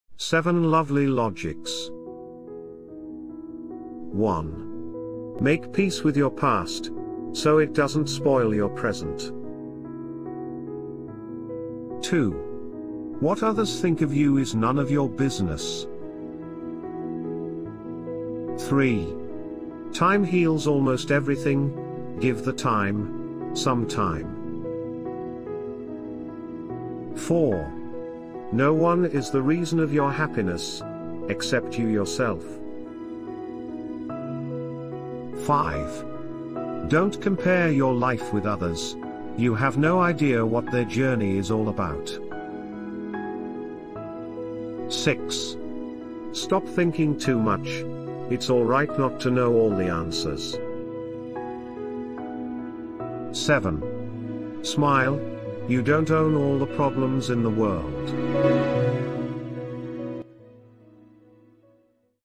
7 Lovely Logics
7 Lovely Logics 1.  Make Peace with your Past so it doesn’t spoil your Present. 2.  What others Think of you is None of your Business. 3.  Time Heals Almost Everything, Give the Time Some Time. 4.  No one is the Reason of your Happiness Except you yourself. 5.  Don’t compare your Life with others, You have No Idea what their journey is all about. 6.  Stop Thinking too much, Its Alright not to know all the Answers. 7.  Smile, you don’t own All the Problems in the World.